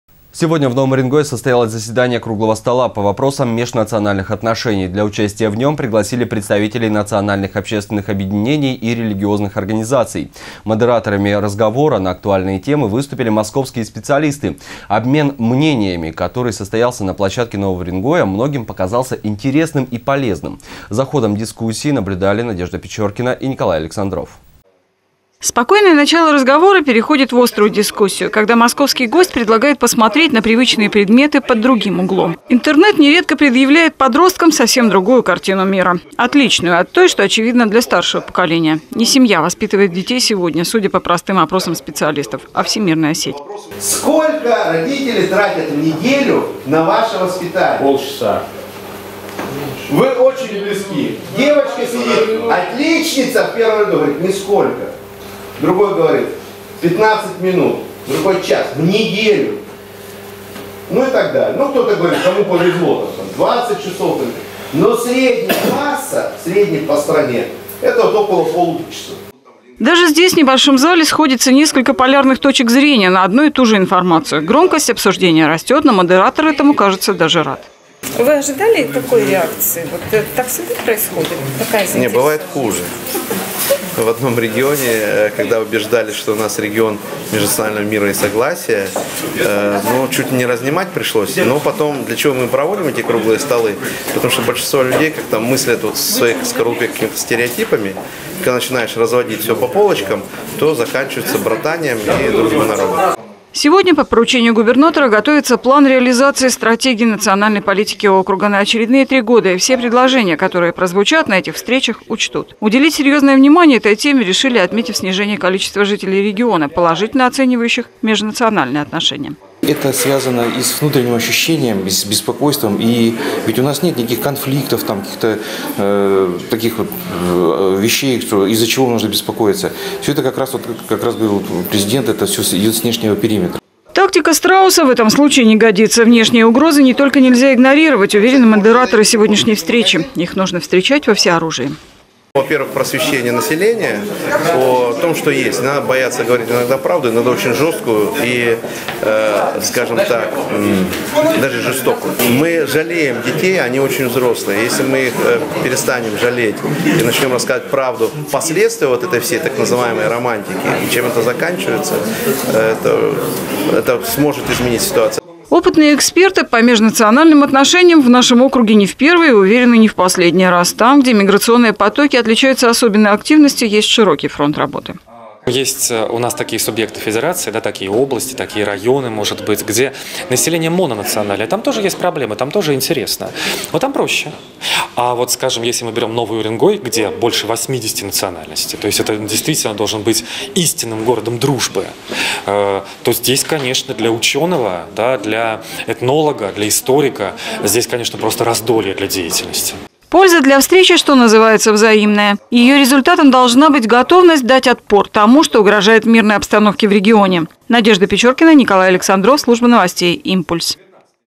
В Новом Уренгое состоялось заседание круглого стола по вопросам межнациональных отношений.
Сегодня в Новом Уренгое состоялось заседание круглого стола по вопросам межнациональных отношений. Для участия в нём пригласили представителей национальных общественных объединений и религиозных организаций. Модераторами разговора на актуальные темы выступили московские специалисты. Обмен мнениями, который состоялся на площадке Нового Уренгоя, многим показался интересным и полезным.